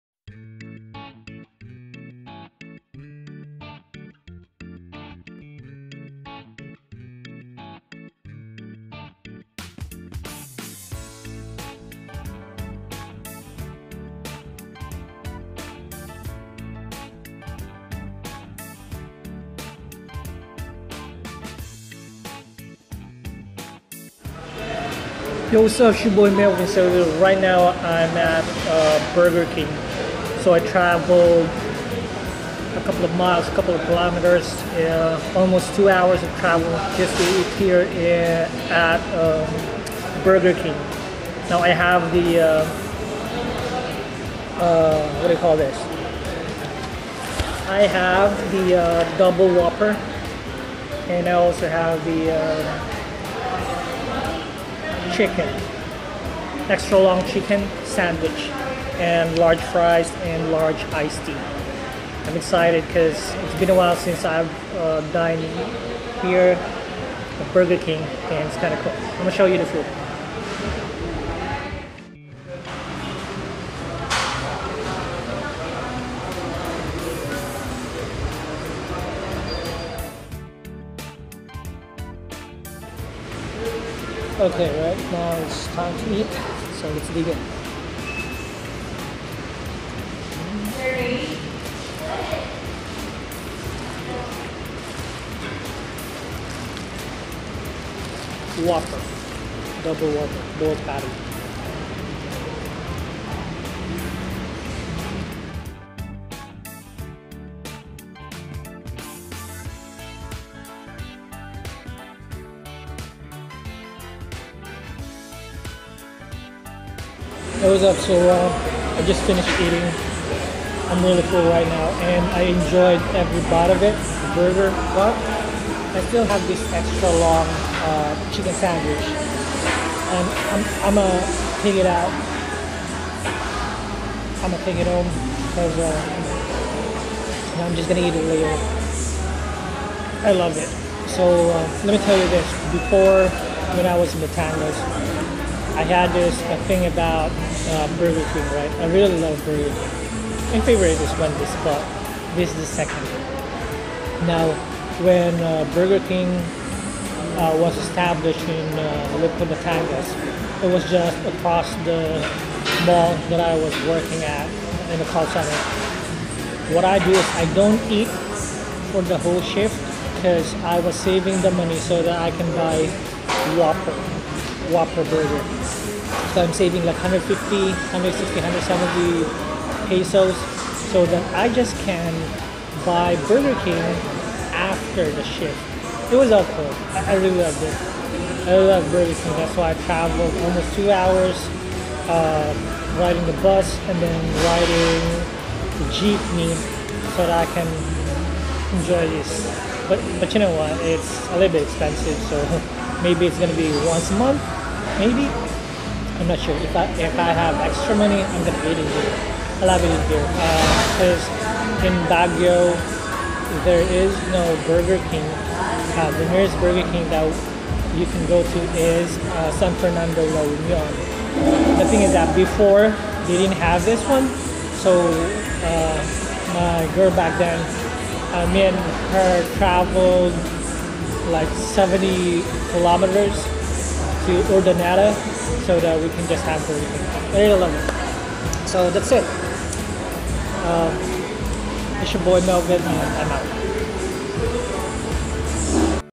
Melvin's Foodtrip #15: Burger King (SFLU)
I travelled more than 100 kilometers just to eat at Burger King.